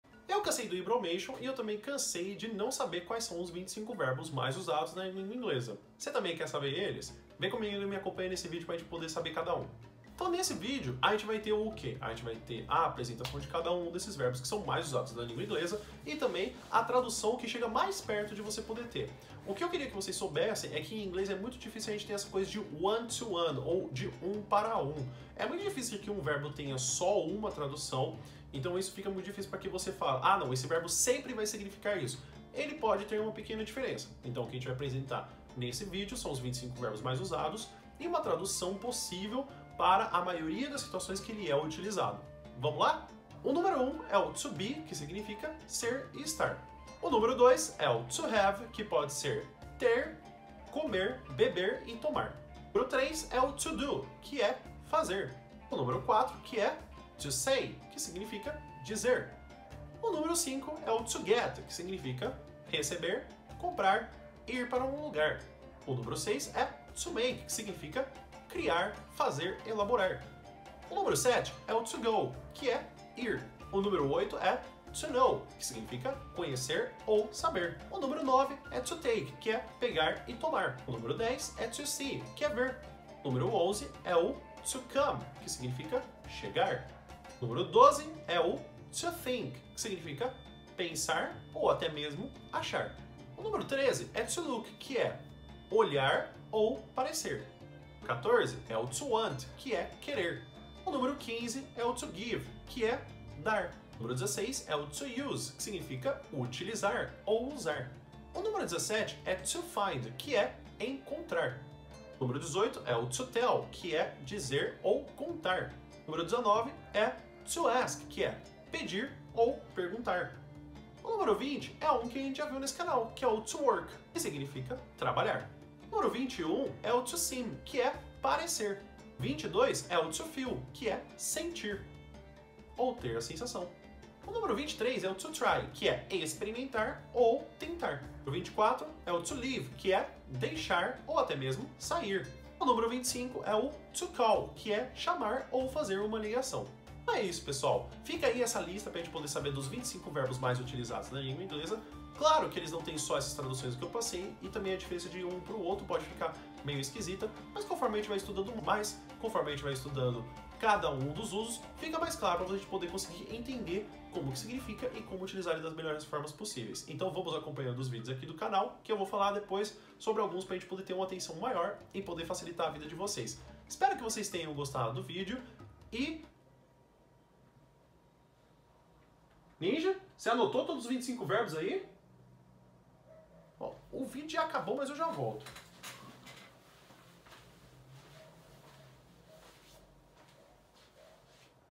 Quais são os 25 verbos mais utilizados na língua inglesa?
1. To Be = ser, estar
2. To Have = ter, comer, beber, tomar
3. To Do = fazer
4. To Say = dizer
5. To Get = receber, comprar, ir
6. To Make = criar, fazer, elaborar
7. To Go = ir
8. To Know = conhecer, saber
9. To Take = pegar, tomar
10. To See = ver, entender
11. To Come = chegar
12. To Think = pensar
13. To Look = olhar, parecer
14. To Want = querer
15. To Give = dar
16. To Use = utilizar, usar
17. To Find = encontrar
18. To Tell = dizer, falar
19. To Ask = perguntar, pedir
20. To Work = trabalhar
21. To Seem = parecer
22. To Feel = sentir, ter a sensação
23. To Try = tentar, experimentar
24. To Leave = deixar
25. To Call = chamar, fazer uma ligação